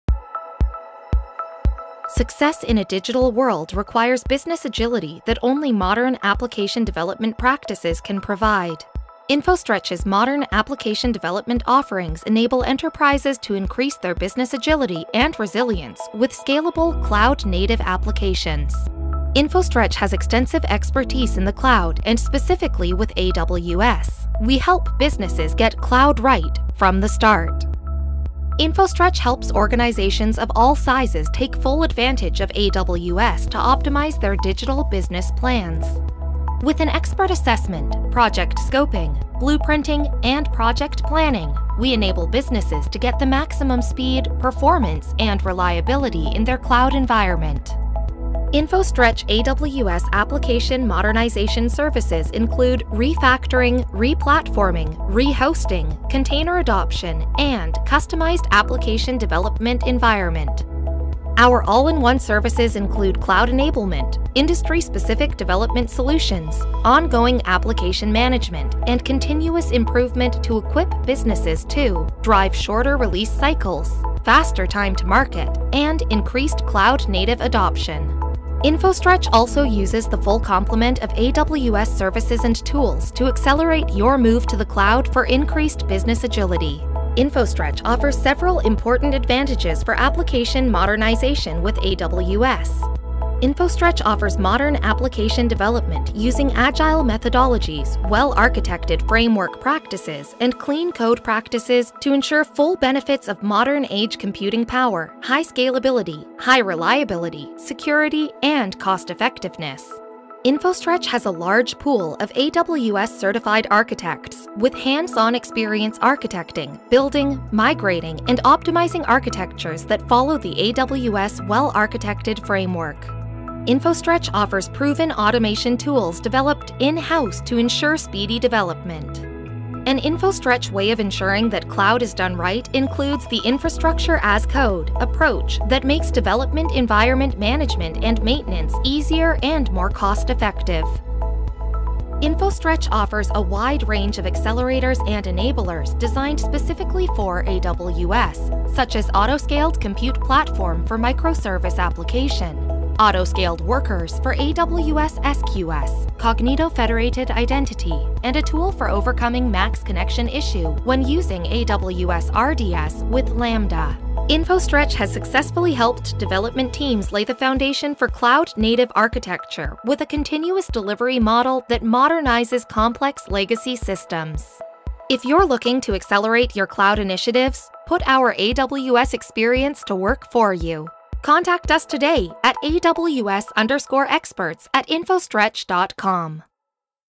Apexon & AWS Application Modernization
No company can become a truly digital business overnight. Taking advantage of cloud native workflow requires the proper modernization approach, one that can account for the challenges of integrating with legacy infrastructure, adopting modern development processes such as DevOps and allowing for continuous delivery. Additionally, you must be able to manage the cultural challenges that come with making these changes.

AWS provides the right technology foundation, but you need the right partner to put that foundation in place and make it work in your specific business and technical environment.